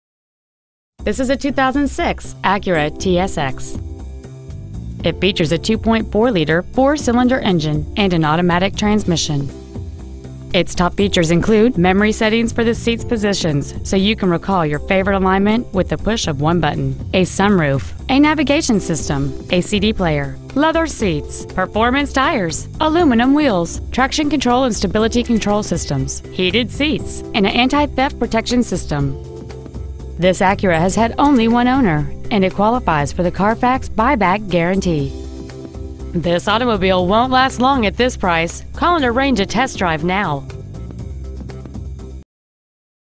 2006 Acura TSX Westmont IL
Year : 2006
 Make : Acura
 Model : TSX
 Engine : 2.4L I4
 Trans . : Automatic
 Exterior : Silver
 Miles : 70,341